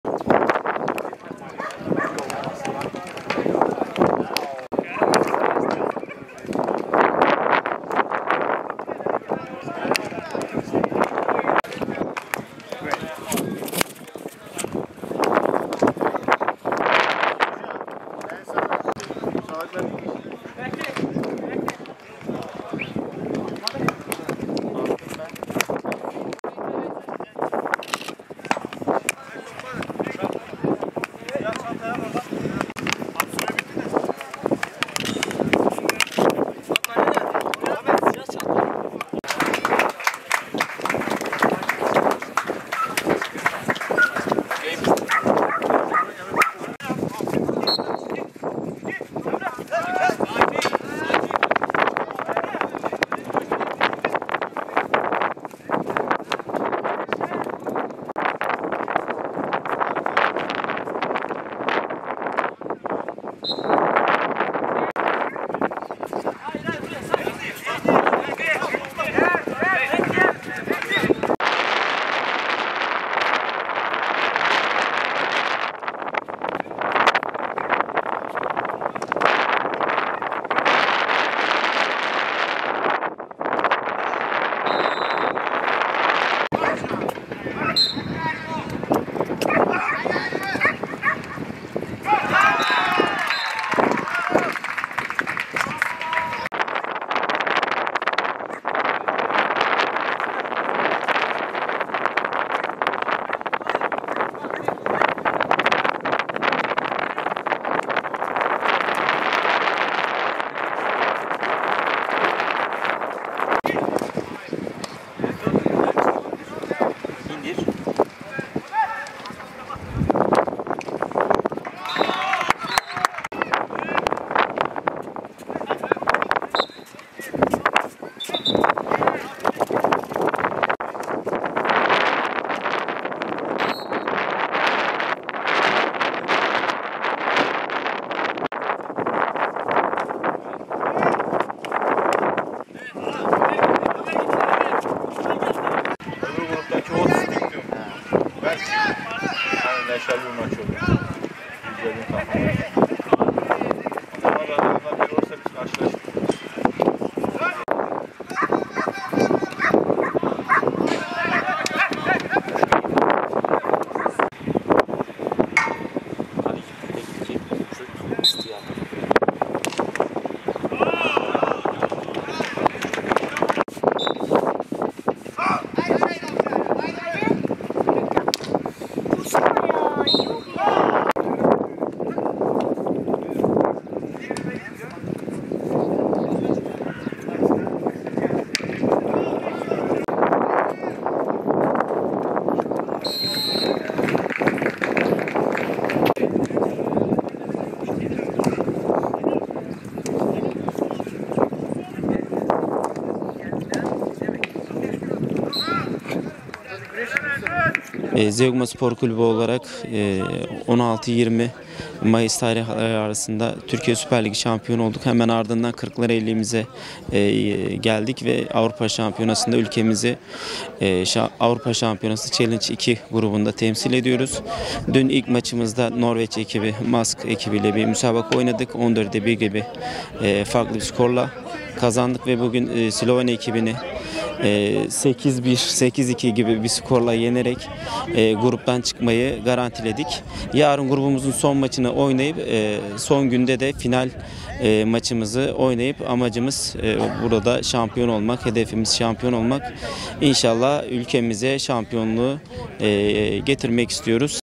Nizip Zeugma, farklı kazandı… 8-2
Kırklareli'nde süren Çim Hokeyi Kulüpler Avrupa Şampiyonası'nda Türkiye'yi temsil eden Gaziantep ekibi Nizip Zeugma, B Grubu maçında Slovenya'nın HK Triglav Predanovci takımını 8-2 yendi.